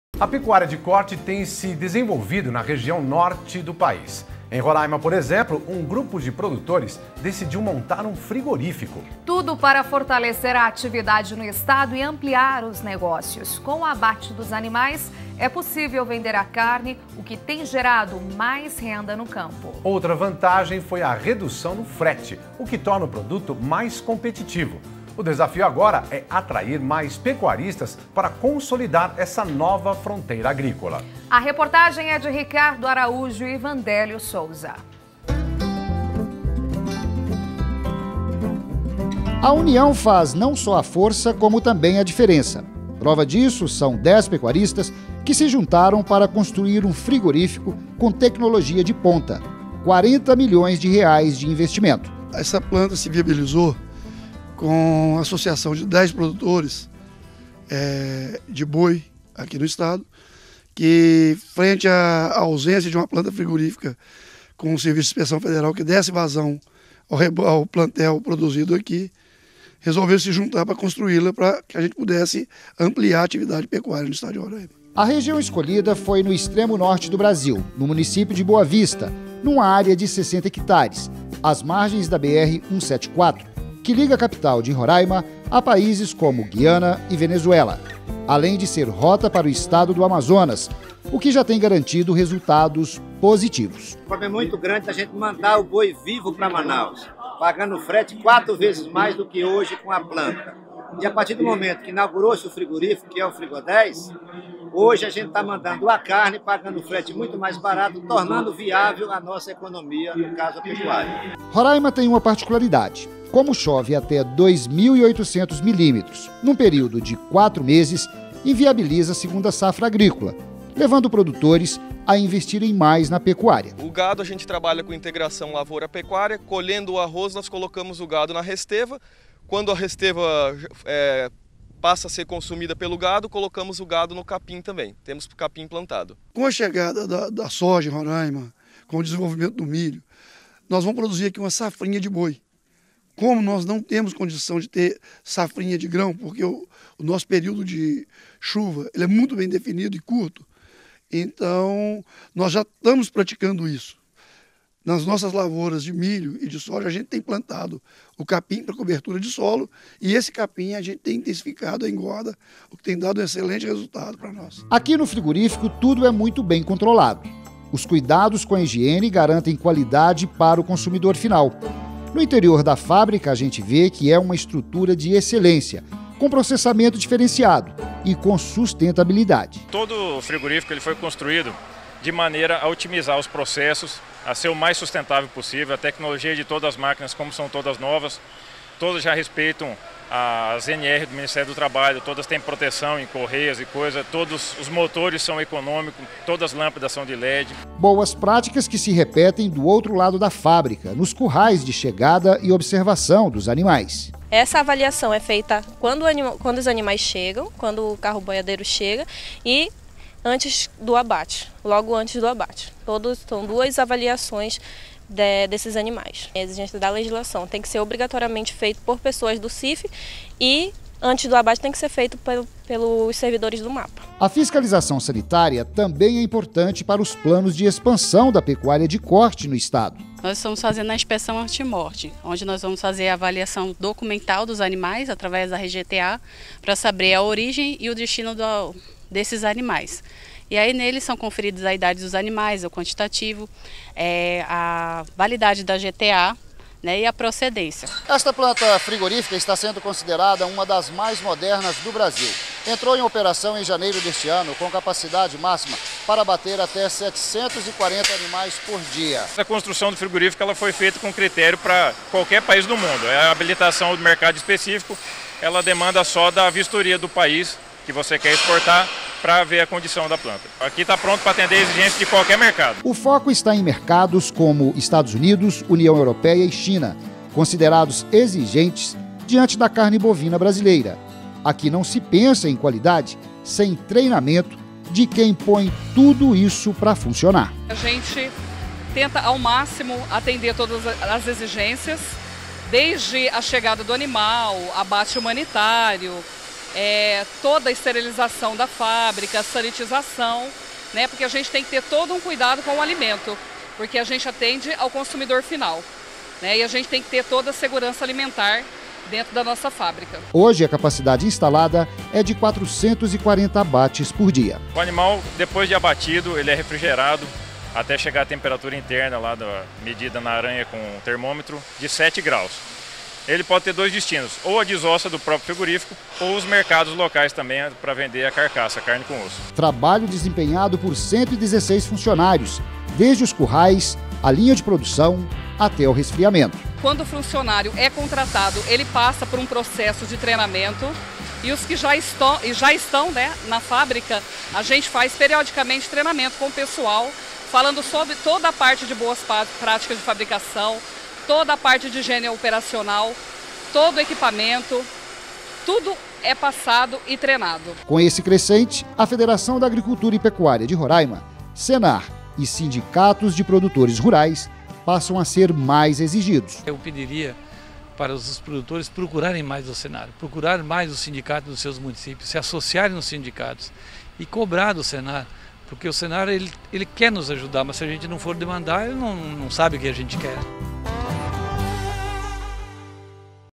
Pecuária de corte se desenvolve em Roraima com apoio do Sistema CNA/Senar e Federação
A pecuária de corte tem se desenvolvido na região norte do país. Em Roraima, um grupo de produtores decidiu montar um frigorífico para fortalecer a atividade no estado e os negócios. Com o abate dos animais, é possível vender a carne, gerando mais renda no campo. Outra vantagem foi a redução no frete, o que torna o produto mais competitivo. O desafio agora é atrair mais pecuaristas para consolidar essa nova fronteira agrícola. Confira!